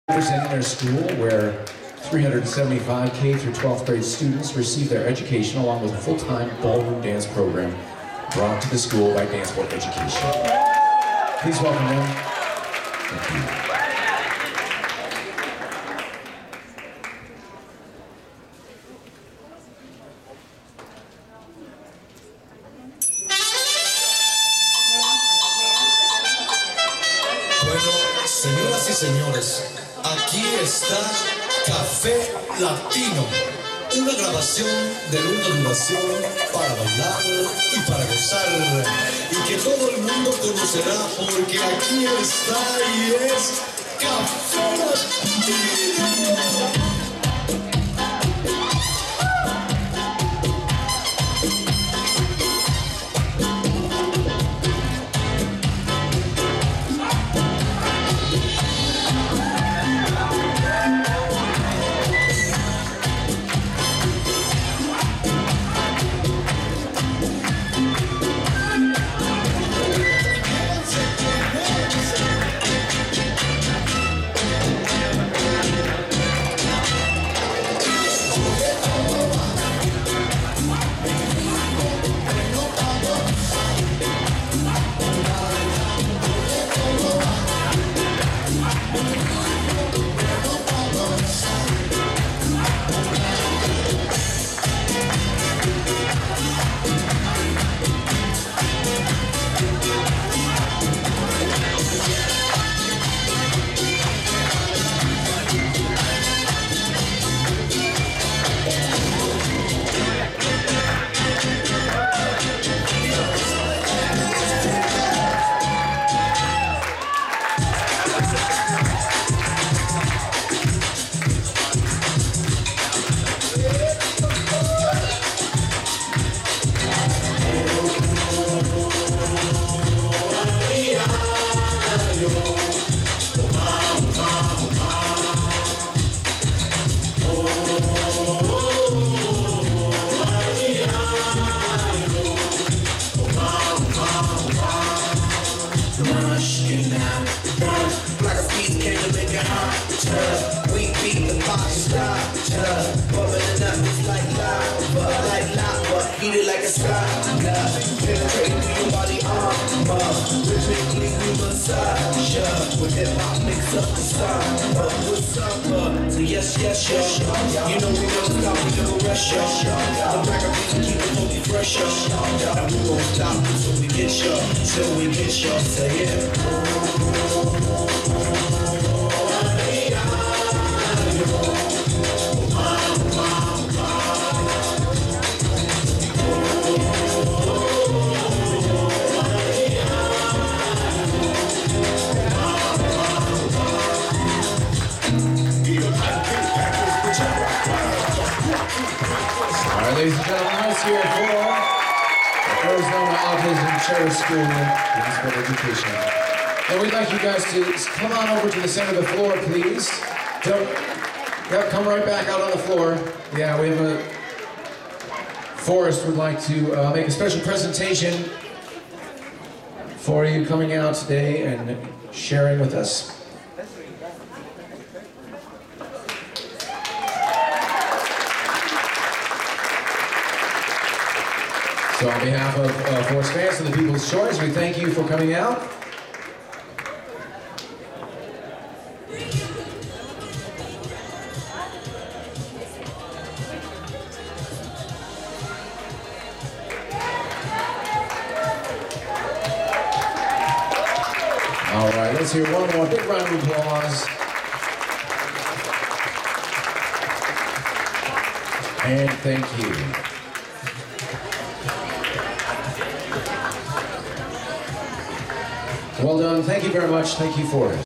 Children Showcase at People's Choice DanceSport Competition
❤️✨AMAZING DANCE TEAM!!! These 5th graders proudly represented their school, Arizona Autism Charter Schoolswhere 375 K-12 grade students receive their education along with a full-time ballroom dance program brought to the school by DanceSport Education. 🧩
❤️✨AMAZING work from Mishka & Oleg Teleha and Ivan Khudoliy & Alisa Seleznova, their top-rated ballroom dance professional instructors, for the short time we put this showcase together! 🧩
❤️✨Our team looks AMAZING in their costumes, special hair and make-up donated by Alan Grundy and Nathan Grundy and their ballroom dance shoes donated by Freed Ballroom USA!!!🧩
❤️✨SHOUT OUT to their parents, family and friends for all the support they provided to make this happen!!!🧩
❤️✨SHOUT OUT to Diana Diaz, the CEO and Founder of Arizona Autism Charter Schools for her love and dedication to the students and to the ballroom dance program!!!🧩
❤️✨SHOUT OUT to Forrest Vance, 7-time US Champion and organizer of People's Choice dancesport Competition for his continuous encouragement and support of our efforts to spread the love of ballroom dance among the Arizona children and youth and promote diversity!🧩
❤️✨ At DanceSport Education, we are proud to bring the benefits of ballroom dance to the students, at their schools, in their classrooms!!!